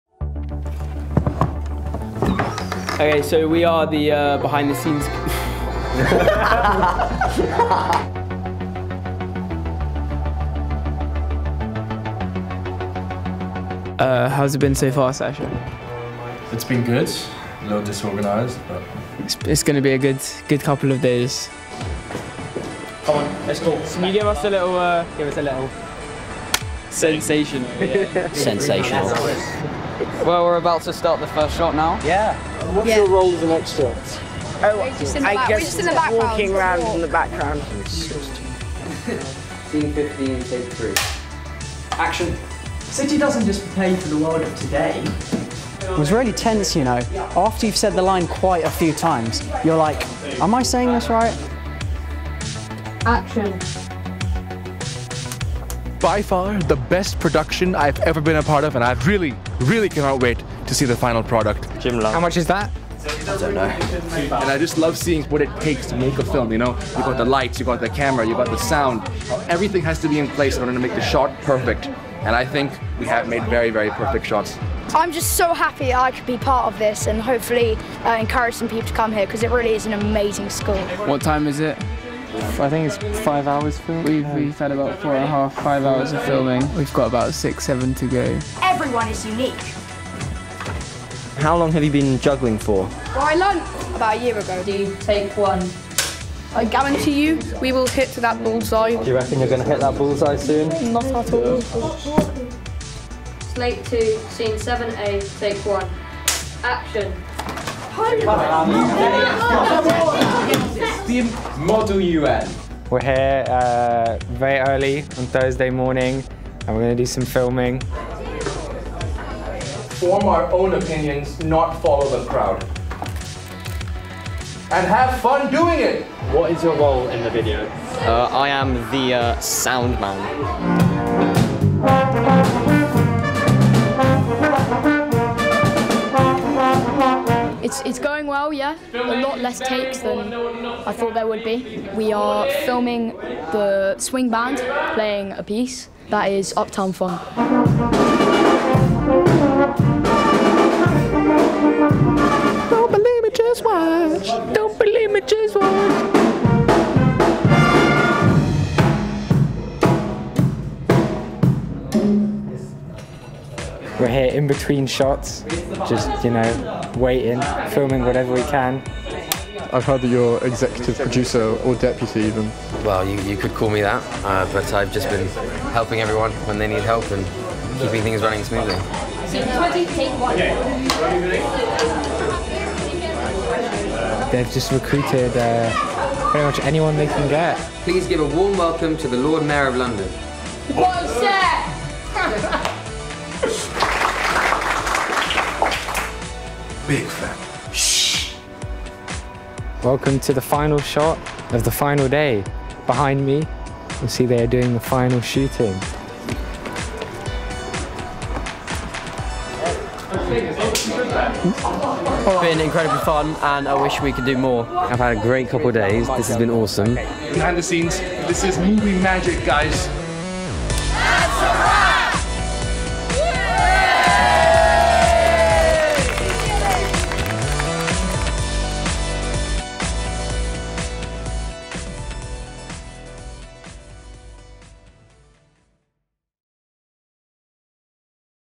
City of London School Prospectus Video - Behind the Scenes
Take a closer look at what went into filming our School Prospectus Video.

Special thanks to the pupils and staff at CLS who helped make this film happen, alongside the Young Film Academy.